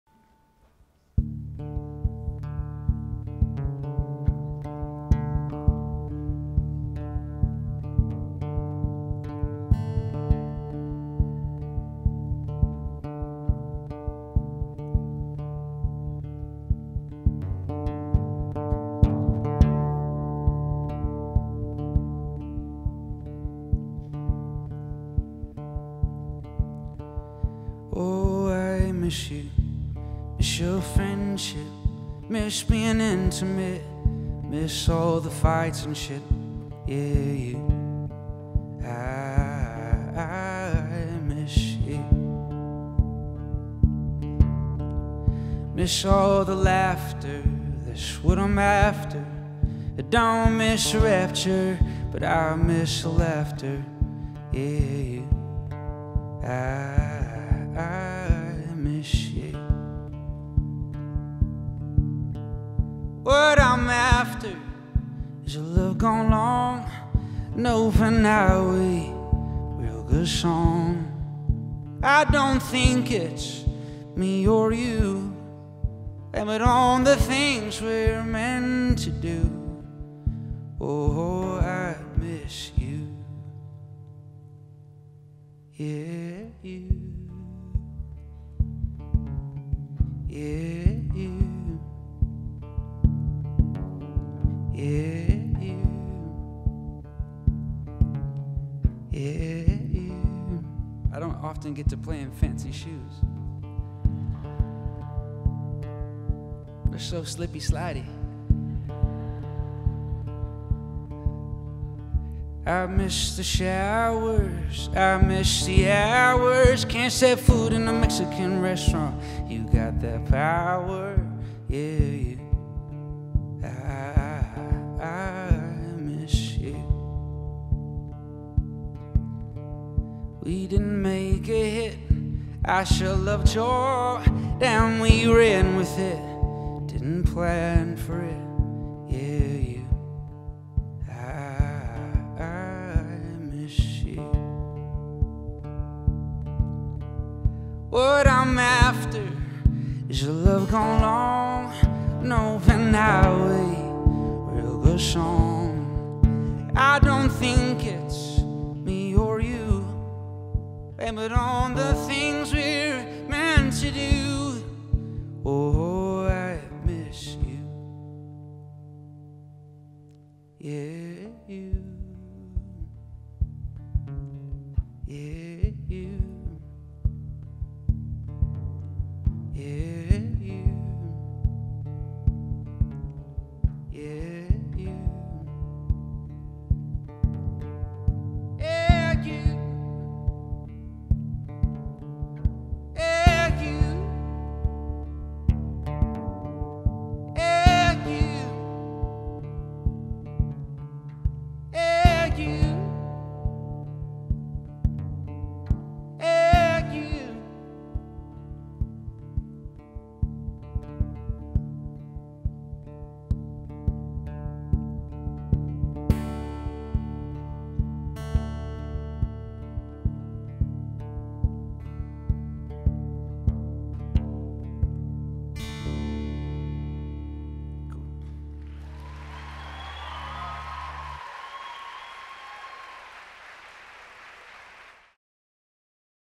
Musical Performance | Michael Bernard Fitzgerald | TEDxYYC
Michael Bernard Fitzgerald (MBF) is a singer, songwriter and proud Calgarian. A touring troubadour, most of MBF’s time is spent on stage playing clubs, theatres and festivals across North America. He is a dog man with a good plan and two hands.
MBF has shared the stage and toured with Joel Plaskett, Michael Franti and Spearhead, Steve Winwood, Sam Roberts, Stars, K-OS, The Wallflowers, Lights and many more. He’s also performed at many a musical festival – including Bonaroo, Firefly Festival and the Calgary Folk Music Festival. This talk was given at a TEDx event using the TED conference format but independently organized by a local community.